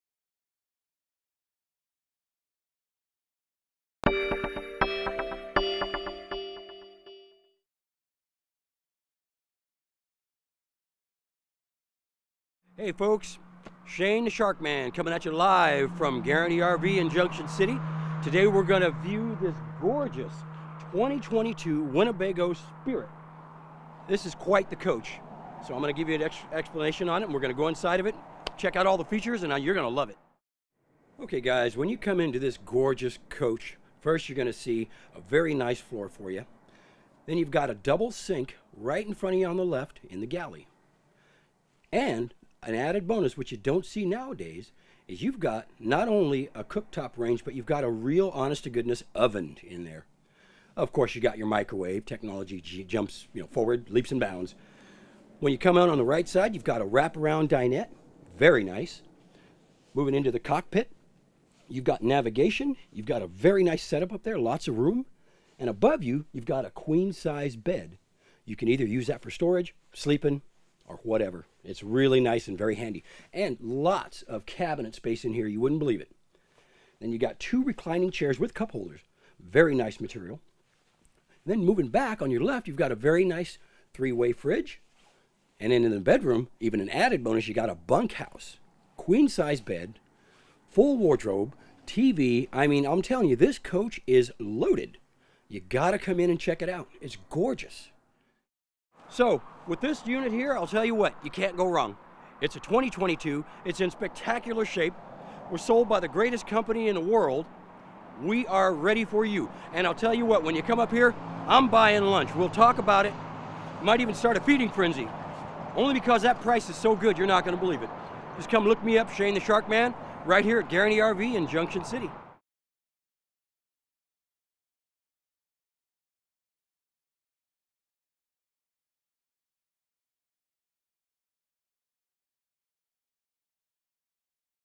2022 Winnebago Spirit 31 H Class C Motorhome • Guaranty.com
Get ready for an adventure in a 2022 Winnebago Spirit 31H. Rest easy in this Class C which comfortably sleeps 10. We've got you covered with: 2 Bunk Bed(s), 2 sofa bed(s) and 1 queen bed. And contains a Bunkhouse. And a master bedroom conveniently located in the Rear. RV Camping in the Spirit Class C offers plenty of at home conveniences like a Center kitchen with U-shaped Dinette, Front living area and Center bathroom. It doesn't stop there, this Spirit 31H Class C intensifies your RV camping experience, and features a stove with 3 oven burners, Full-Size refrigerator, 2 TVs, and more. This Spirit 31H, has a GVWR of 14500 lbs . It also offers 1 Black Water Holding Tank(s), 1 Fresh Water Holding Tank(s), and 1 Gray Water Holding Tanks. This Spirit also contains 1 door(s), 1 awning(s), 2 slideout(s).